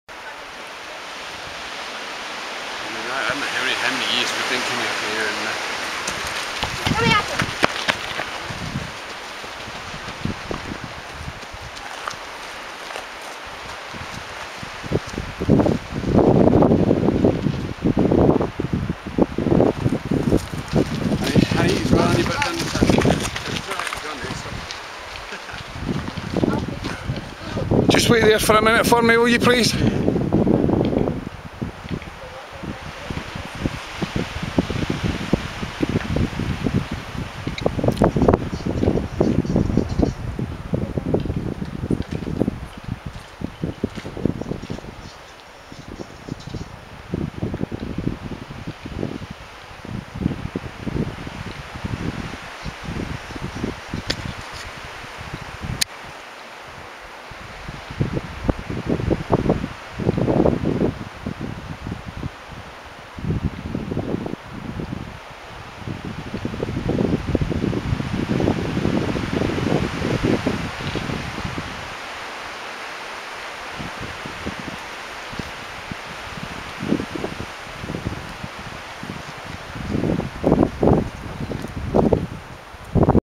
VinnieRothiemurchusCapercailzie 2.AVI
"Vinnie was the rogue Rothiemurchus capercailzie who guarded HIS path through the forest near Aviemore!  Sadly killed by a dog not under control in mid-April 2012 thus robbing us all of the sight of this rare and magnificent bird in full "lek" mode.